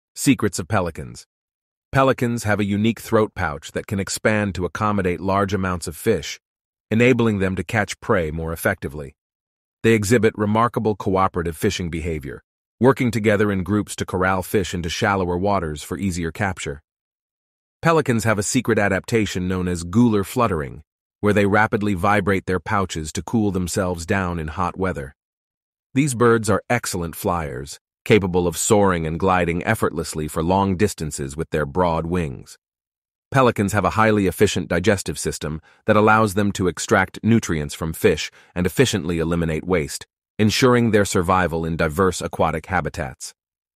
Pelican's Unique Facts 🔥
secrets of pelicans🔥

Pelicans possess intriguing secrets including their expandable throat pouches, cooperative fishing behavior, gular fluttering for cooling, adept flying skills, and efficient digestive systems, making them fascinating and adaptable birds of the aquatic world.